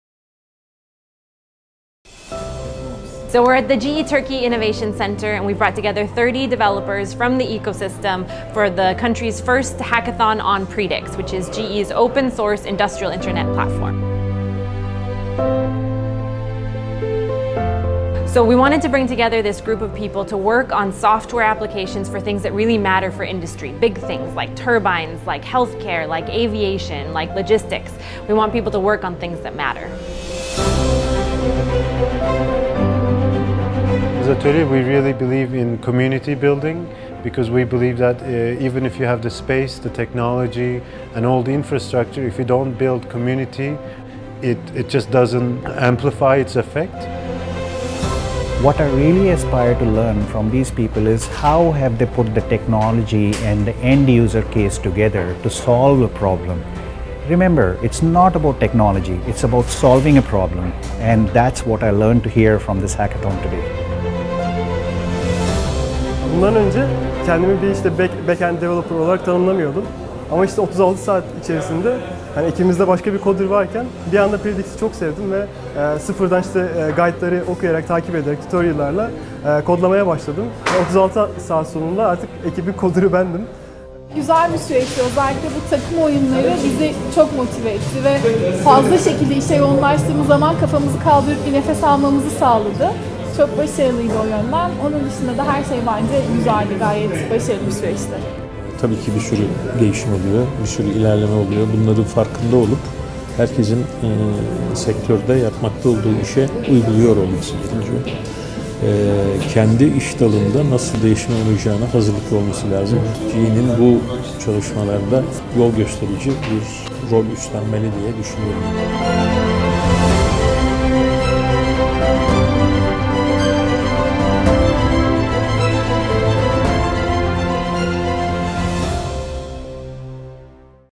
GE Hosts Turkey’s First Industrial Internet Hackathon on Predix
GE hosted a group of 30+ Turkish entrepreneurs and developers in the country’s first Industrial Internet hackathon. The group were challenged to use the Predix platform to create a digital solution focused on Turkey’s critical automotive industry based on real data